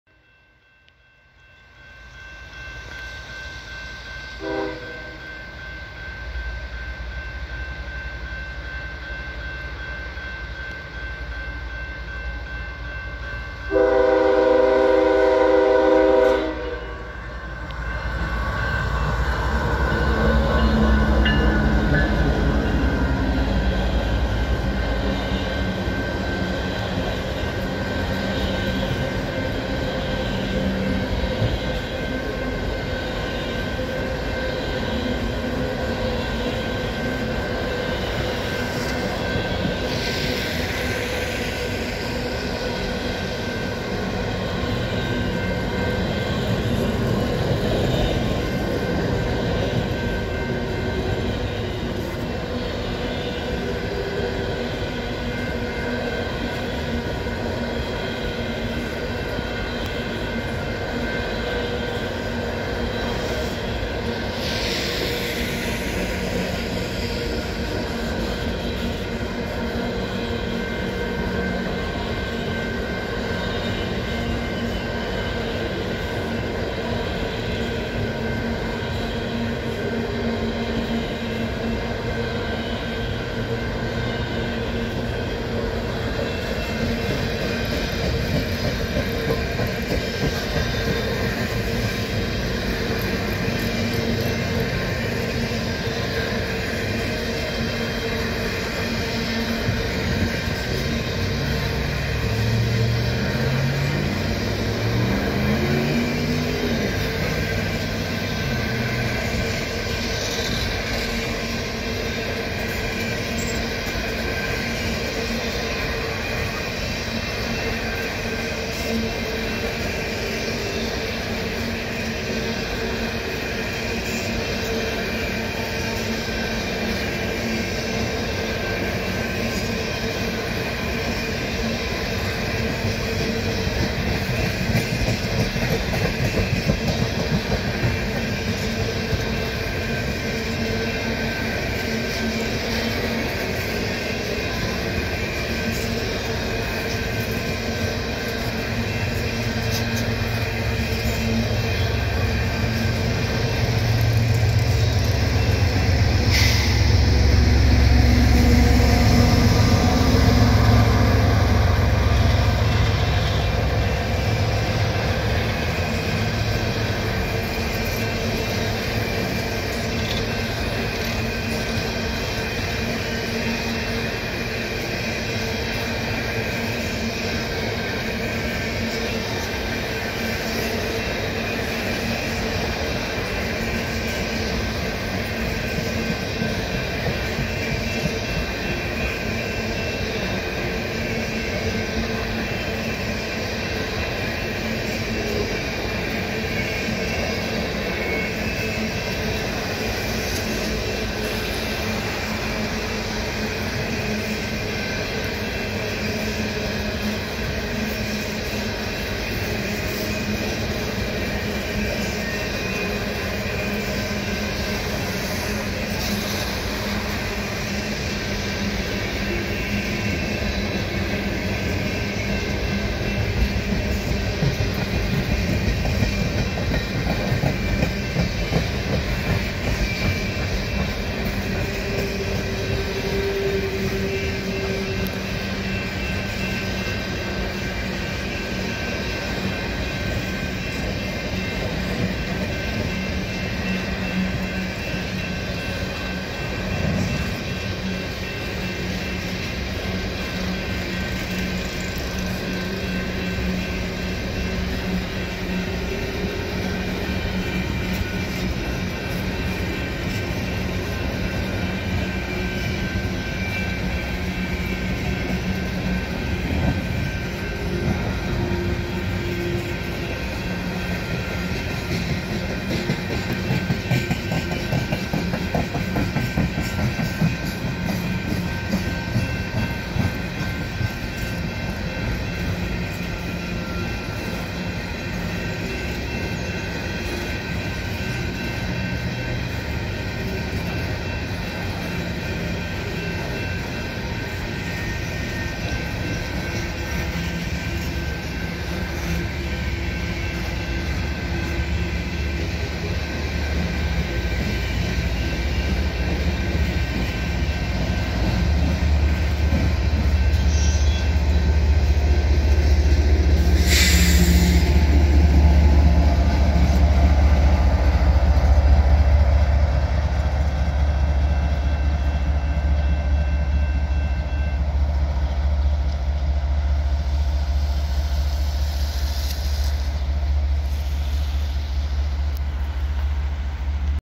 GEVO Power! CP 8764 Leads Coal Hopper Train w/DPU'S In Matsqui British Columbia
Filmed at Riverside St grade crossing, Canadian Pacific's Page Sub in Matsqui British Columbia.

CP 8764 GE GEVO ES44AC leads coal hopper train with DPU'S CP 8959 GE GEVO ES44AC and CP 8700 GE GEVO ES44AC.